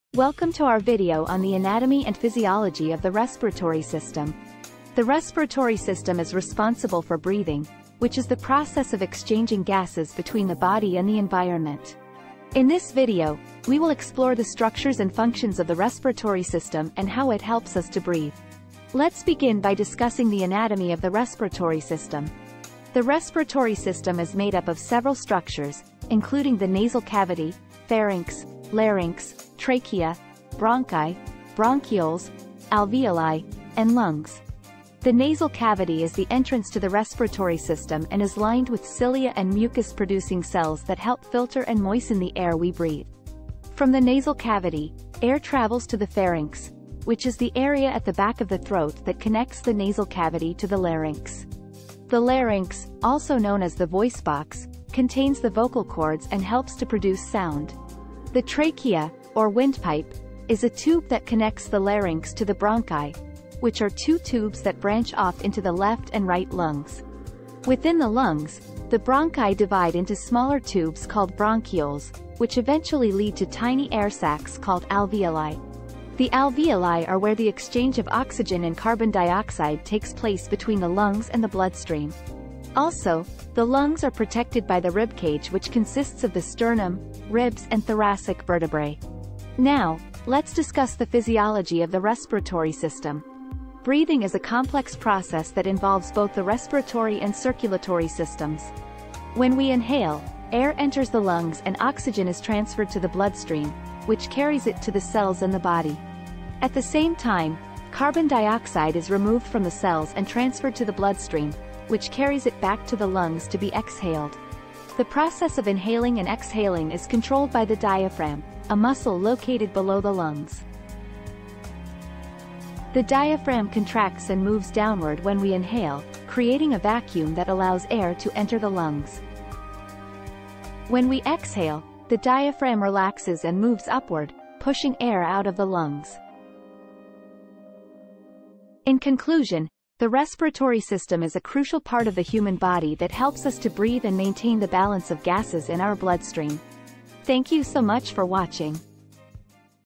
Learn about Respiratory System in 2 minutes
In this video, we explore the anatomy and physiology of the respiratory system, which is responsible for breathing and exchanging gases between the body and the environment. We discuss the structures of the respiratory system, including the nasal cavity, pharynx, larynx, trachea, bronchi, bronchioles, alveoli, and lungs. We also delve into the physiology of breathing and how oxygen and carbon dioxide are exchanged between the lungs and bloodstream. By the end of this video, you'll have a better understanding of the complexity and importance of the respiratory system in our everyday lives.